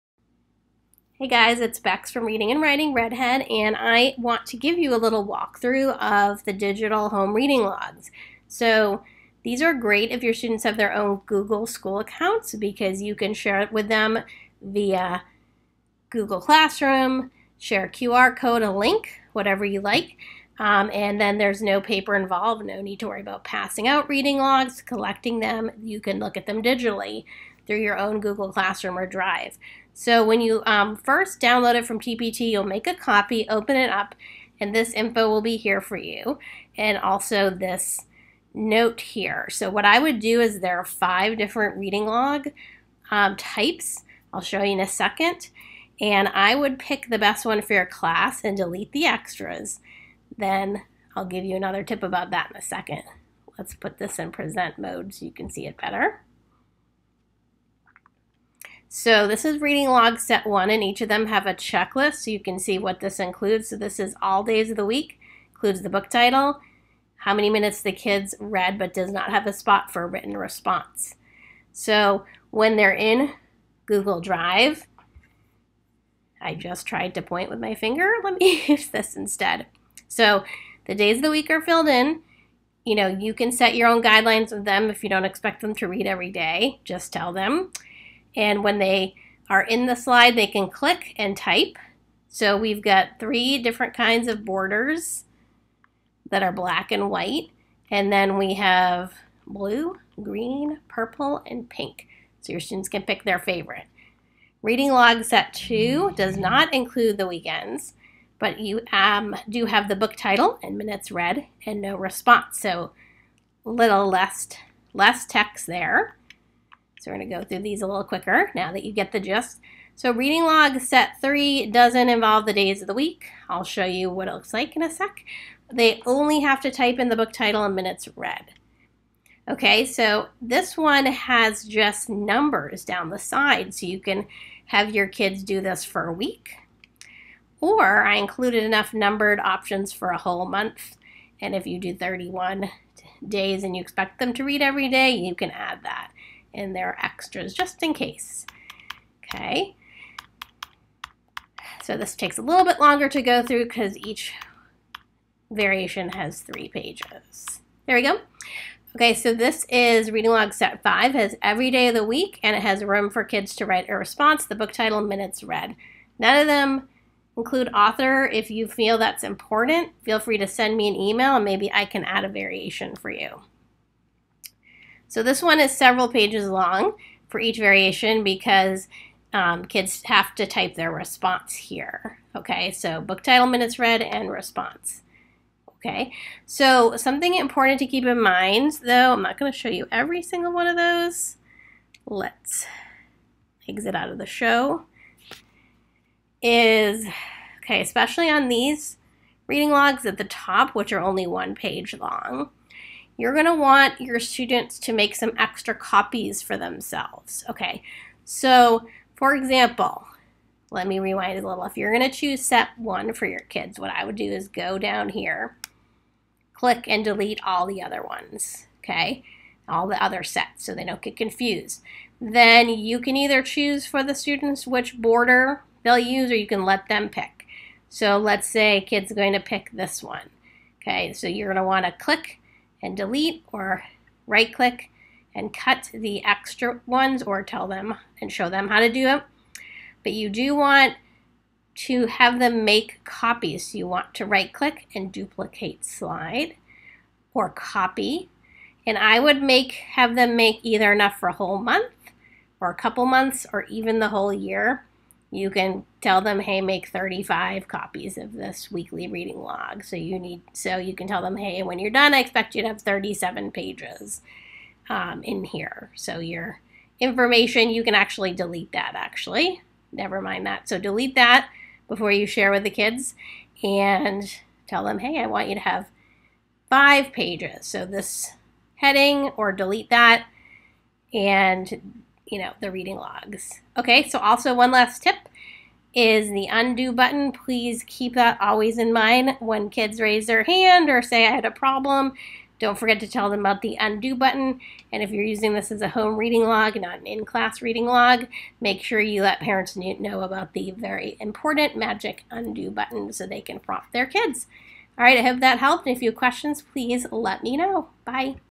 Walkthrough of the Digital Home Reading Logs for use with Google
Does printing, copying, passing out, and collecting reading logs give you a headache?! If your students have Google school accounts and access to Google Drive or Google Classroom, these digital reading logs could be just the thing for you! It comes in 5 versions and each version comes with a variety of borders so you or your students can choose their favorite.